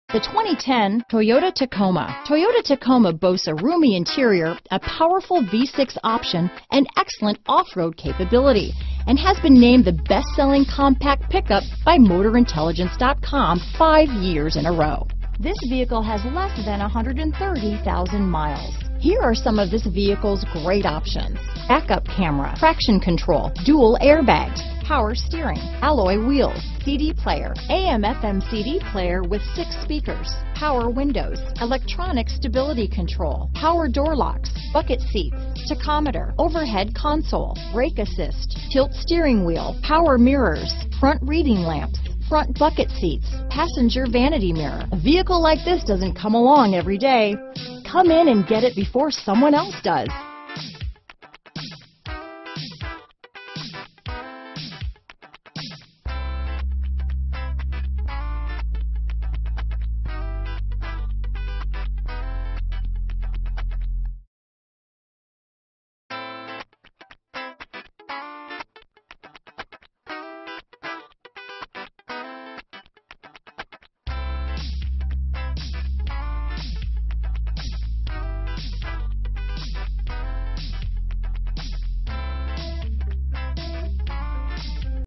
2010 Toyota Tacoma Easton, Allentown, Bethlehem, Phillipsburg and Stroudsburg PA T44621B
U 2010 Toyota Tacoma
2010 Toyota Tacoma  - Stock#: T44621B - VIN#: 5TEMU4FN0AZ698434

Koch Toyota
3816 Hecktown Rd

NEW TIRES, CLEAN CARFAX...NO ACCIDENTS, INCLUDES PRE-PAID MAINTENANCE, BACK UP CAMERA, ALLOY WHEELS, 4WD, ABS brakes, Electronic Stability Control, Traction control.brbrbrAwards:br  * 2010 KBB.com Best Resale Value AwardsbrKOCH 33 MEANS MORE!